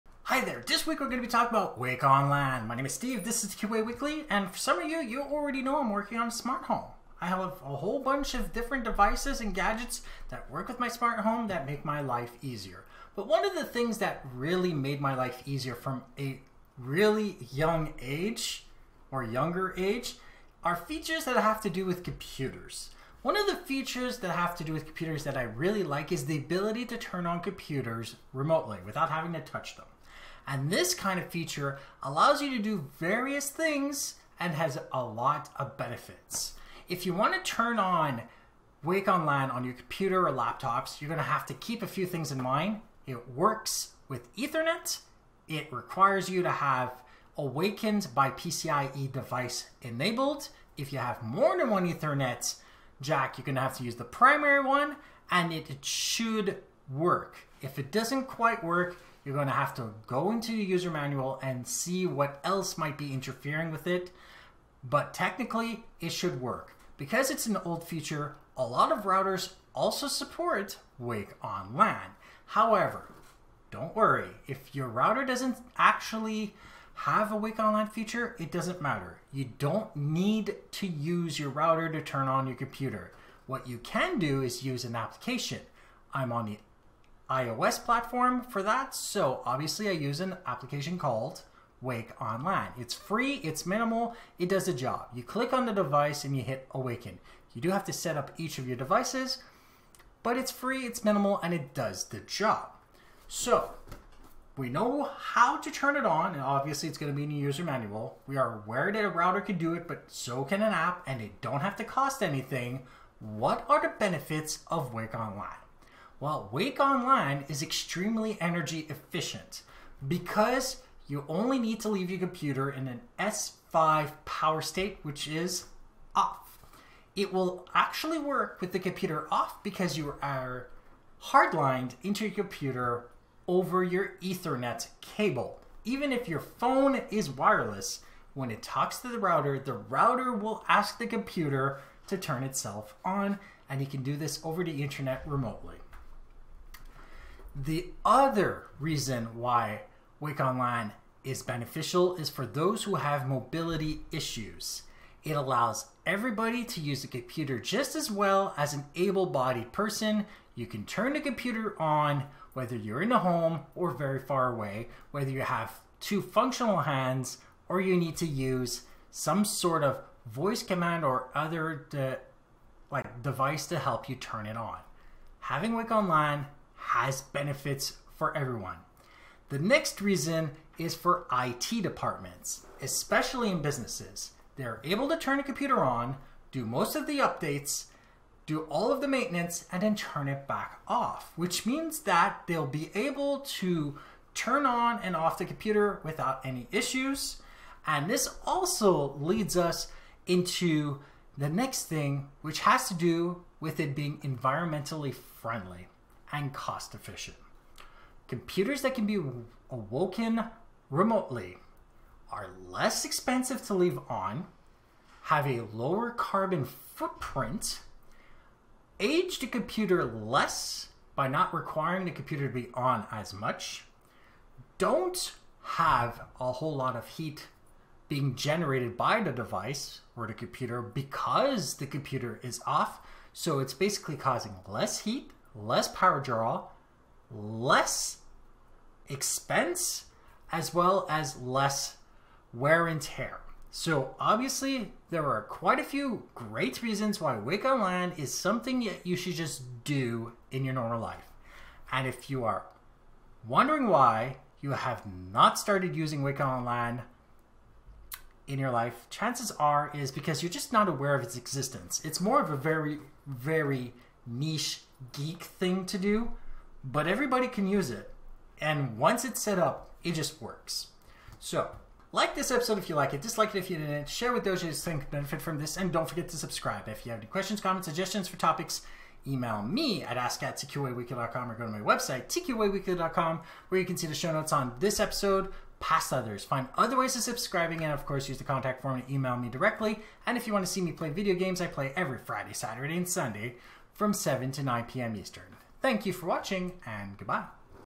What is Wake-On-Lan?
Learn about the feature Wake-On-Lan and all of its benefits.